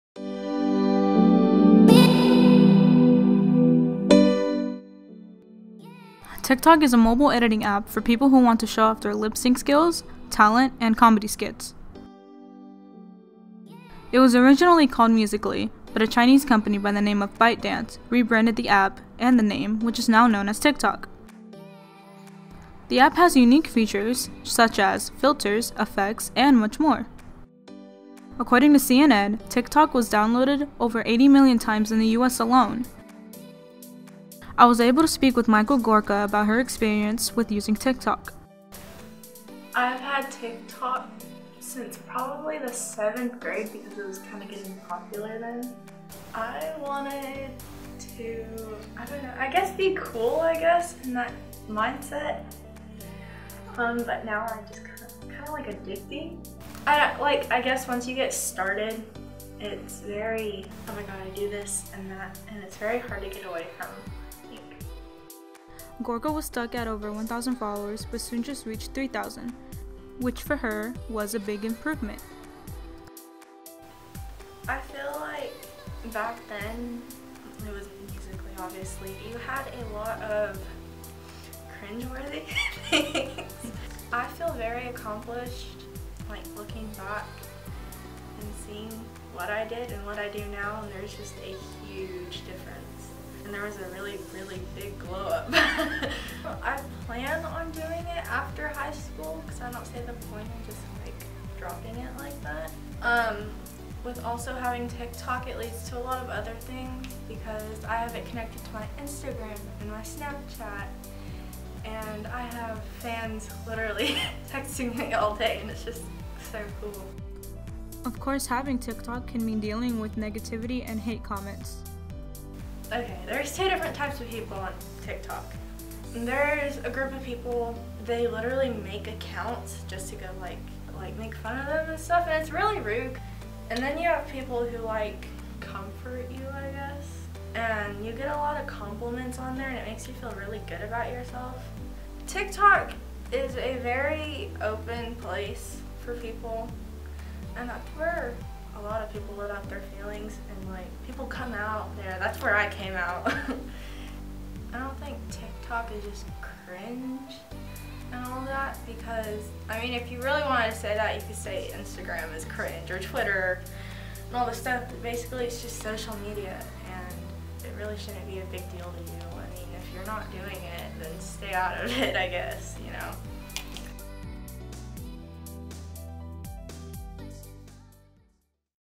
East High Messenger Volume 126 Issue 2
Michael Lynn Gorka makes TikTok videos during her free time. In this video, she will briefly talk about her experience with using the mobile editing app.

Video shot and edited by: Guadalupe Hernandez

The Messenger Video works in part of East High's "The Messenger" newspaper staff.